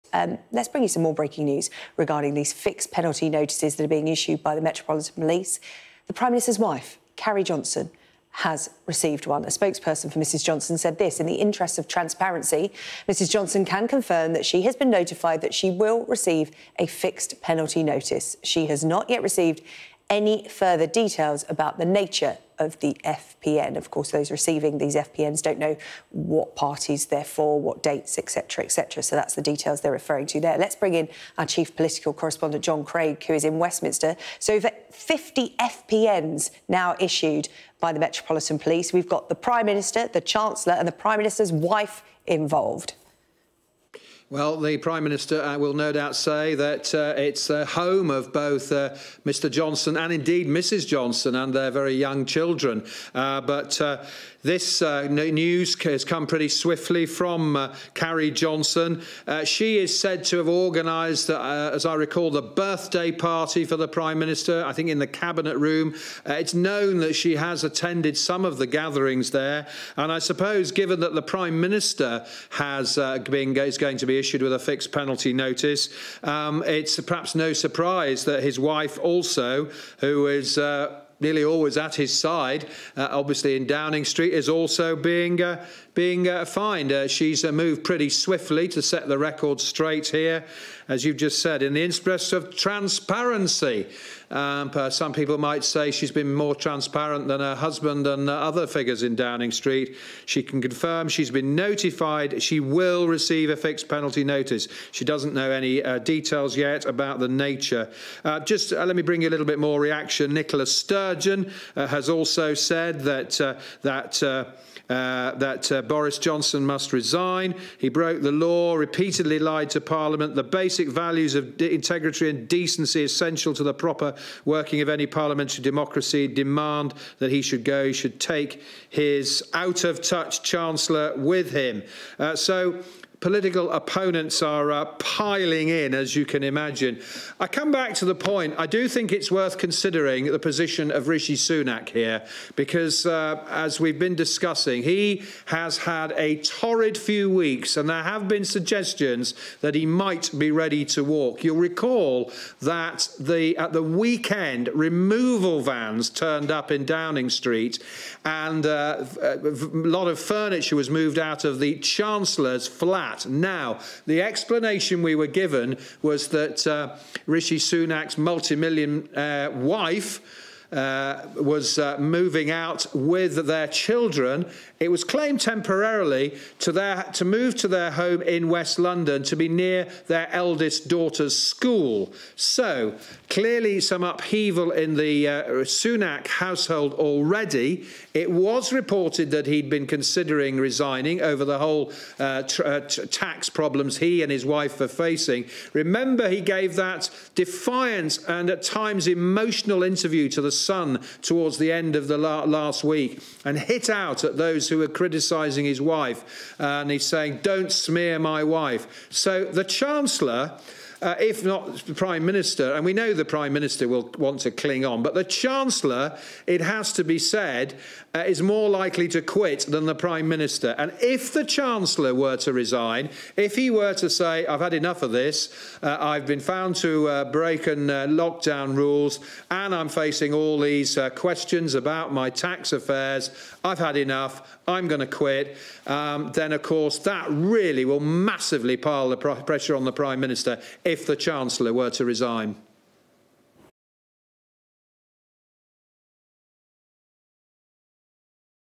Carrie Johnson to be fined over parties
Boris Johnson and his wife Carrie have been notified that they will be issued with fines for breaking lockdown rules.

Rishi Sunak has also received a fixed penalty notice as a  result of a Metropolitan Police investigation into parties in Downing Street and Whitehall in 2020 and 2021.